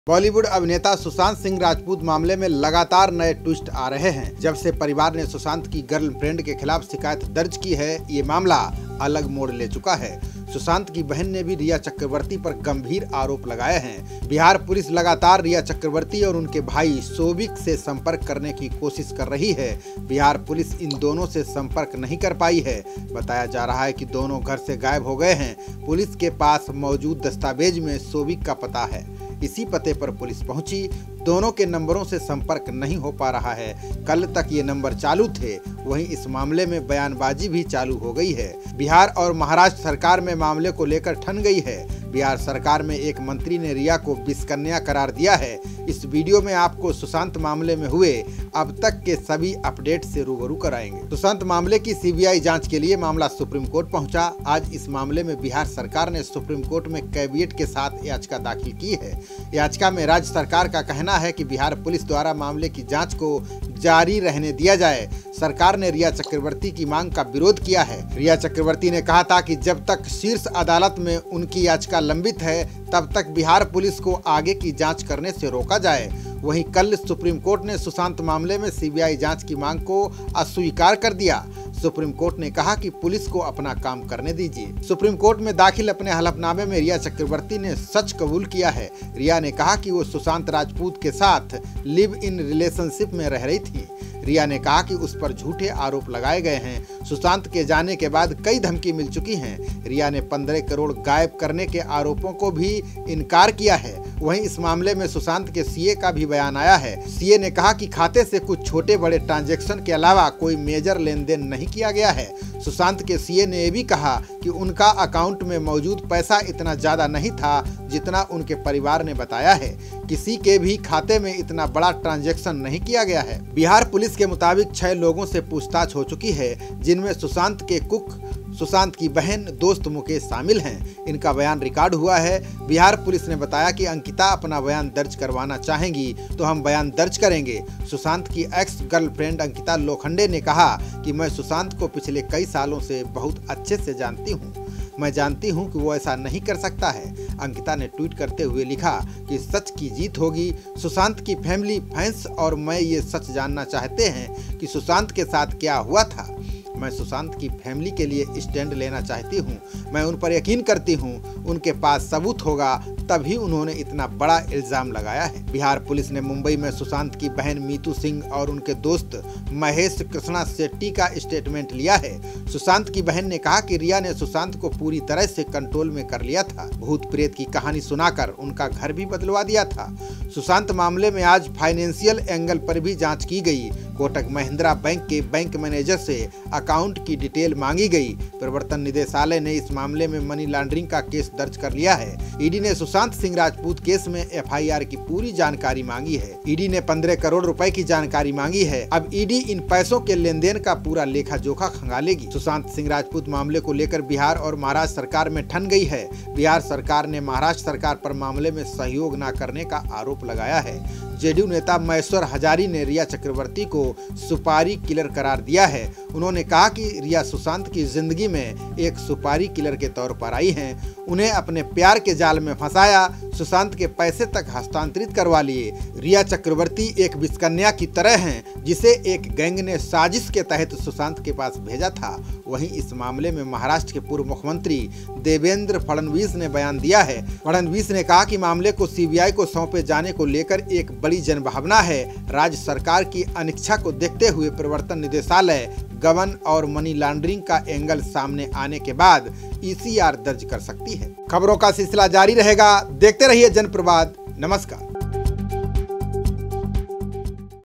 Sushant Case : Rhea Chakraborty ने स्वीकारी सच्चाई | 15 करोड़ हेराफेरी की ईडी ने शुरू की जांच
सुशांत मामले में आ रहे हैं नए ट्विस्ट
सुशांत की बहन ने रिया पर लगाए गंभीर आरोप
बिहार पुलिस कर रही रिया की तलाश
रिया और उसका भाई घर से गायब
दोनों के नंबरों से नहीं हो पा रहा संपर्क
सीबीआई जांच के लिए मामला पहुंचा सुप्रीम कोर्ट
बिहार सरकार ने लगाई कैविएट और याचिका
बिहार पुलिस की जांच को रहने दिया जाए जारी
सुप्रीम कोर्ट ने सीबीआई जांच को किया अस्वीकार
रिया चक्रवर्ती ने कुबूल किया सच
सुशांत के साथ लिव इन में रह रही थी रिया
रिया ने 15 करोड़ के आरोपों से किया इनकार
बिहार पुलिस 6 लोगों से कर चुकी है पूछताछ
सुशांत की एक्स गर्लफ्रेंड अंकिता का बयान
सुशांत की बहन मीतू का लिया स्टेटमेंट
ईडी ने दर्ज किया मनी लांड्रिंग का केस
15 करोड़ मामले की जांच करेगी ईडी
बिहार और महाराष्ट्र सरकार में ठनी
महाराष्ट्र सरकार पर सहयोग न करने का आरोप
महेश्वर हजारी ने रिया को बताया सुपारी किलर
रिया को हजारी ने बताया विष कन्या
फडणवीस ने उठाई सीबीआई जांच की मांग